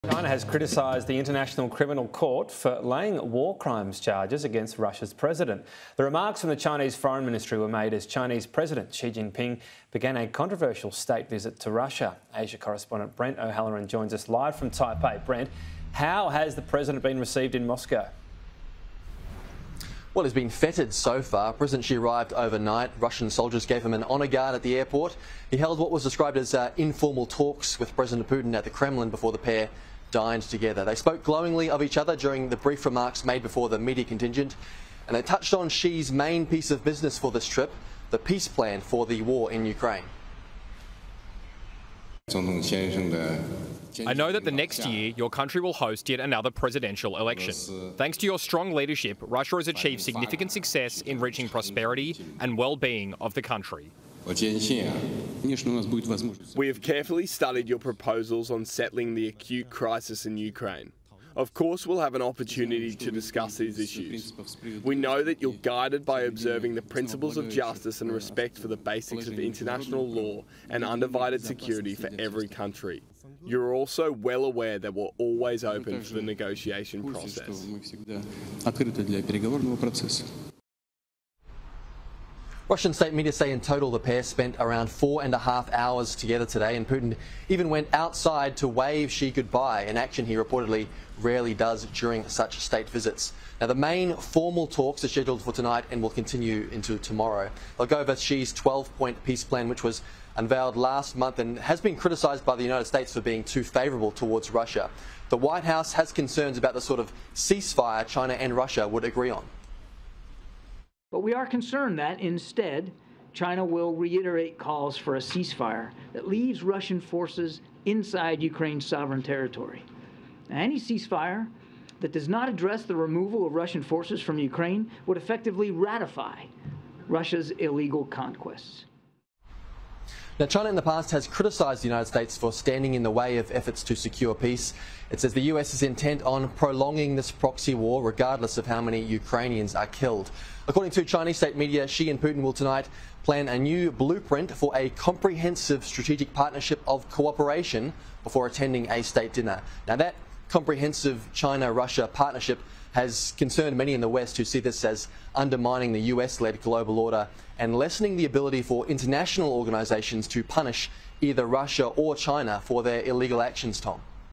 China criticizes International Criminal Court for charges against Putin
China has criticised the International Criminal Court for criminal war charges against Russia’s president Vladimir Putin as Chinese President Xi Jinping visited Russia.

The Chinese President had informal talks with President Putin and touched on one of the main reasons for the visit involving the peace plan in Ukraine.

President Jinping’s trip to Russia has been controversial, however, both presidents have spoken glowingly about each other.